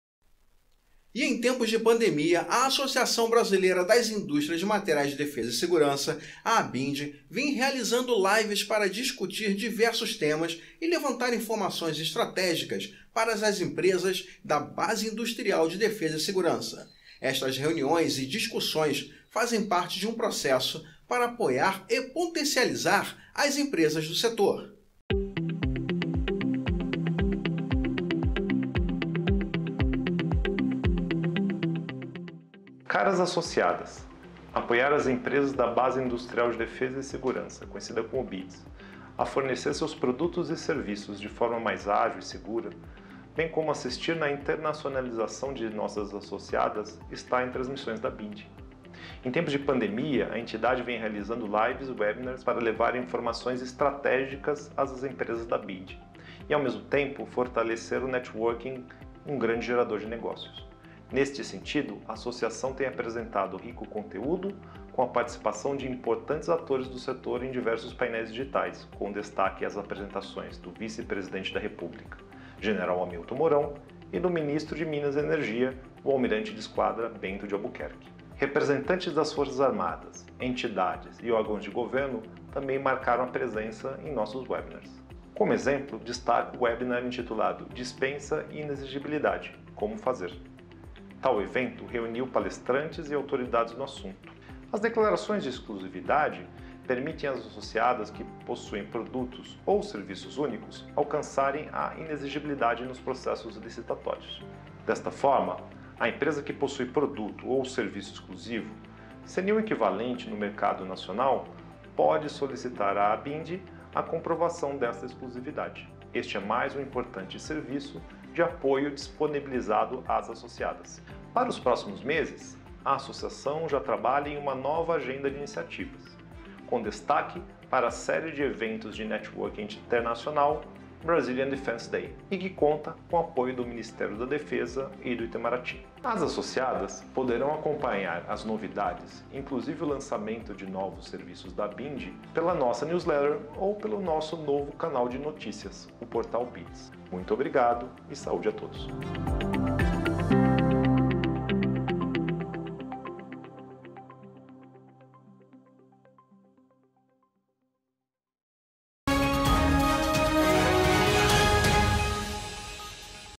ABIMDE realiza webinars para discutir temas para empresas da Base Industrial de defesa e Segurança
Em tempos de pandemia, a Associação Brasileira das Indústrias de Materiais de Defesa e Segurança - ABIMDE vem realizando lives e webinars para discutir diversos temas e levar informações estratégicas para as empresas da Base Industrial de defesa e Segurança. Estas reuniões e discussões fazem parte de um processo para apoiar e potencializar as empresas do setor.

Apresentação: Luiz Camões
Créditos: Exército Brasileiro

Muito obrigado.